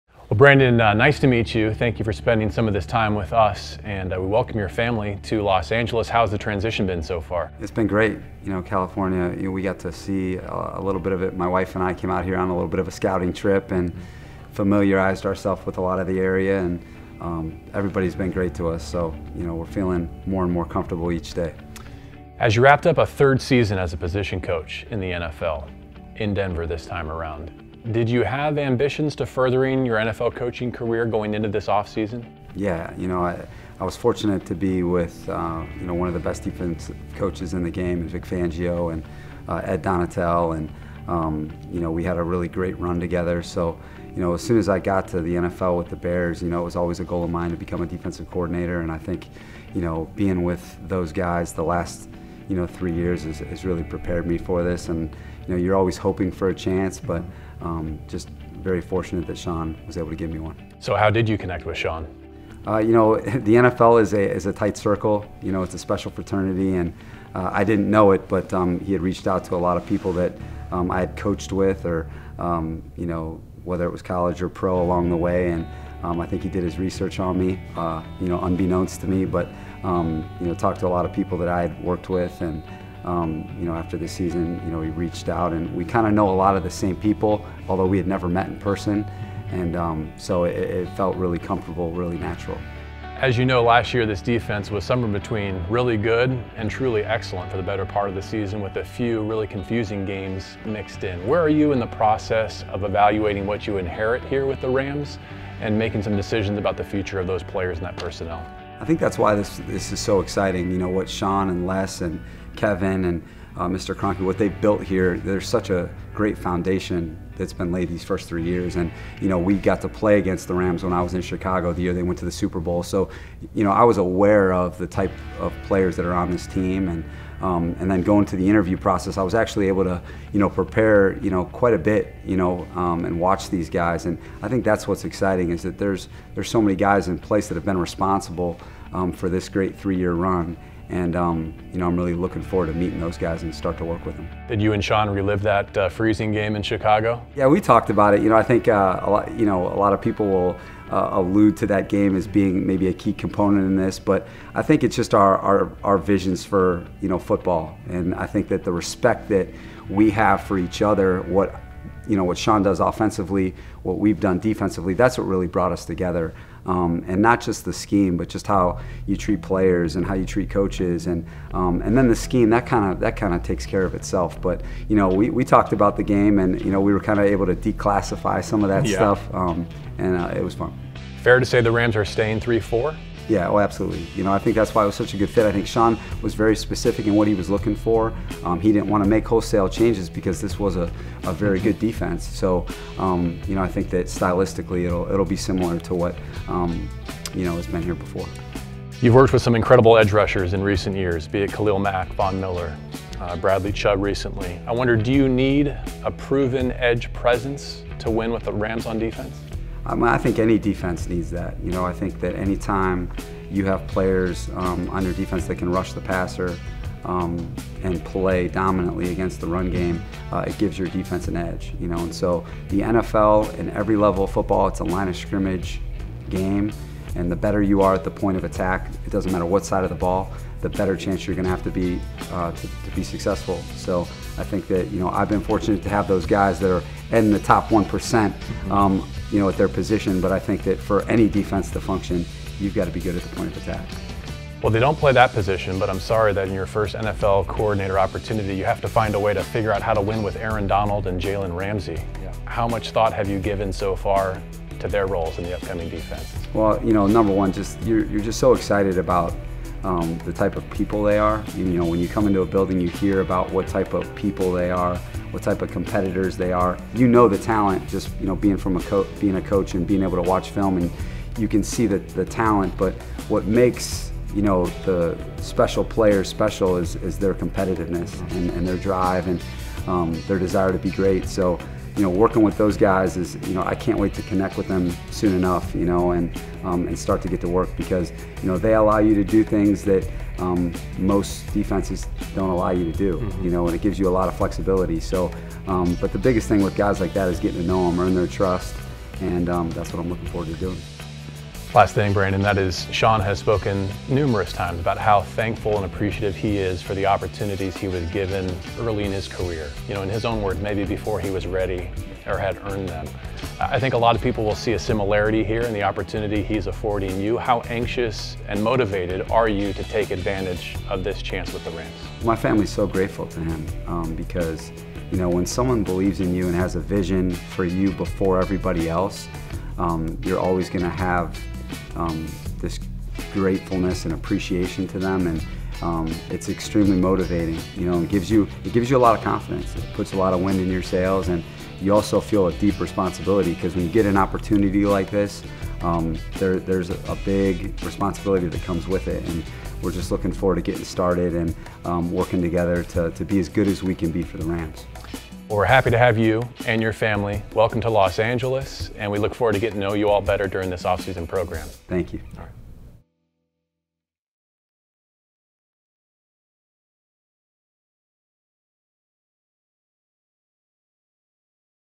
"Gotta Win at the Point of Attack" Rams New DC Brandon Staley Plans to Stick With 3-4 Defense
The Rams new defensive coordinator sits down with J.B. Long and shares his thoughts on how this defense has been successful in previous years and how he plans to utilize Aaron Donald and Jalen Ramsey.